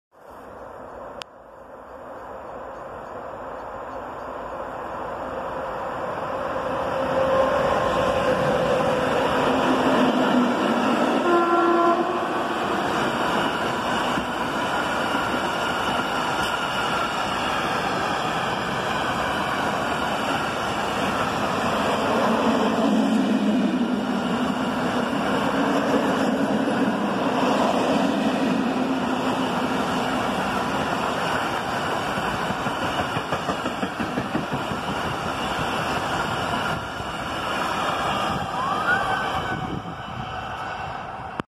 Eurosprinter z pakietem beczek
Około godziny 18 przy przejeździe kolejowym przy ulicy Jasna Rola w Poznaniu przelatuje eurosprinter ze składem beczek z jednym podkutym wagonem .